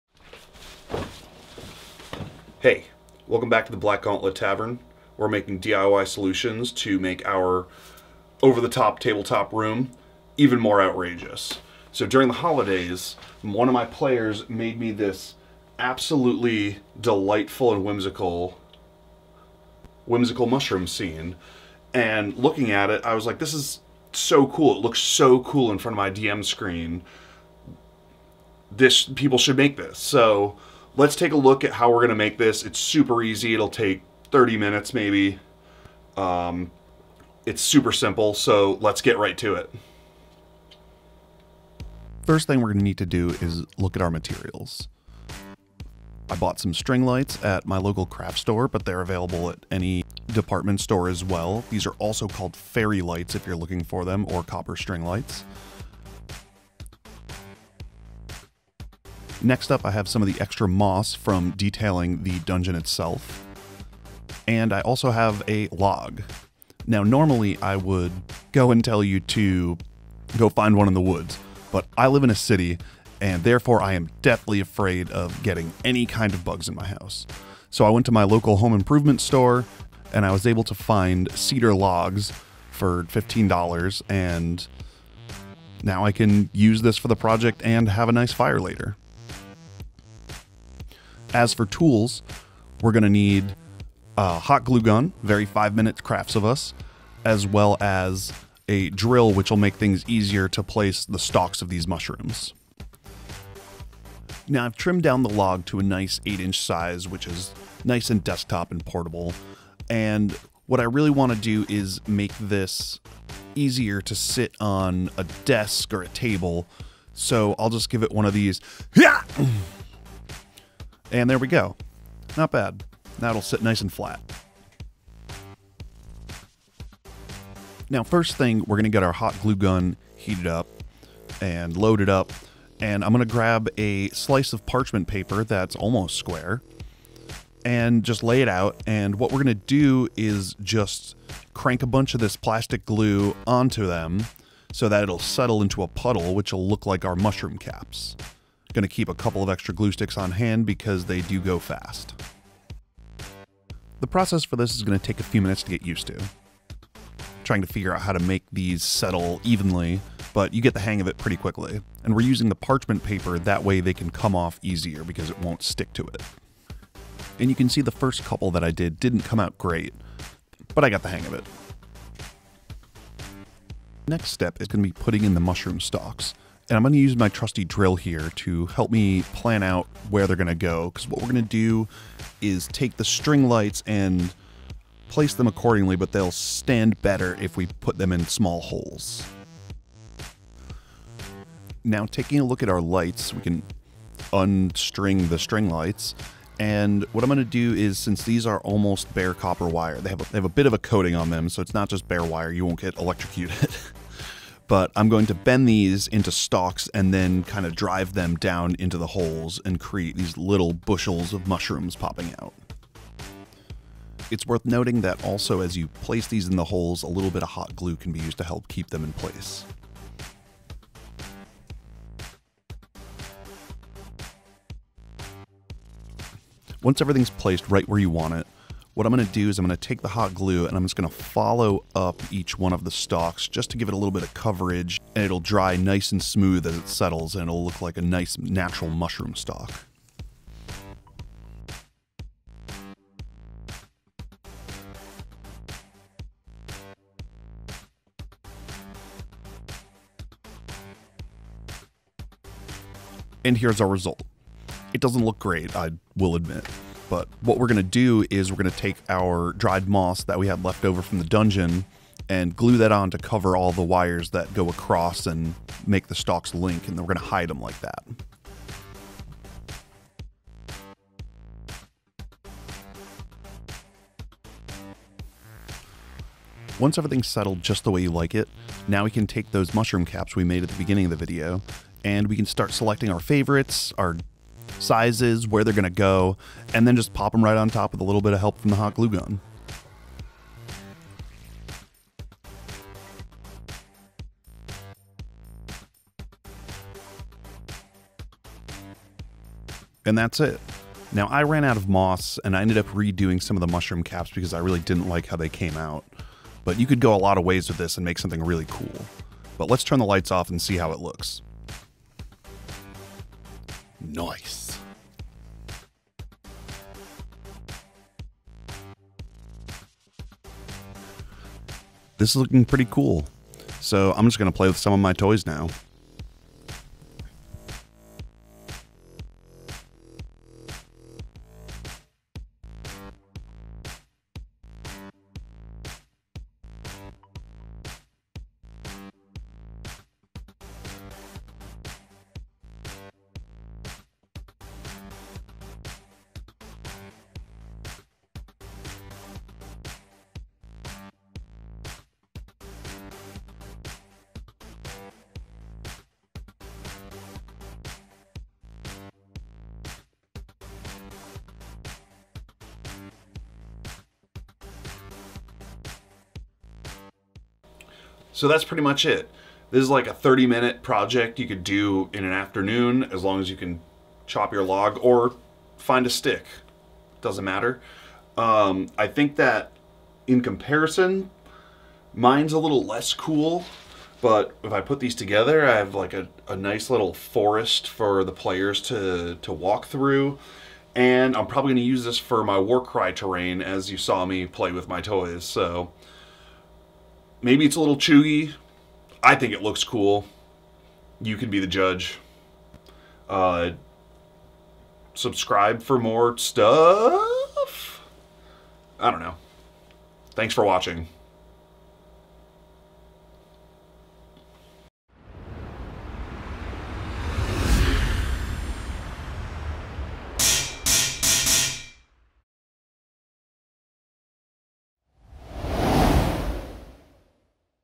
Less than $40 Whimsical Mushroom Set-Piece! || Dungeon DIY || The Back Gauntlet Tavern
Re-creating a gift from a player, a fantastic mystical mushroom set piece! Lets get real crafty and make a little decoration for our tavern!

Materials:
Copper string lights /faerie lights - $10
Birch logs - $15
Faux/ dried moss - $10

Tools:
Hot glue gun
Drill
A good attitude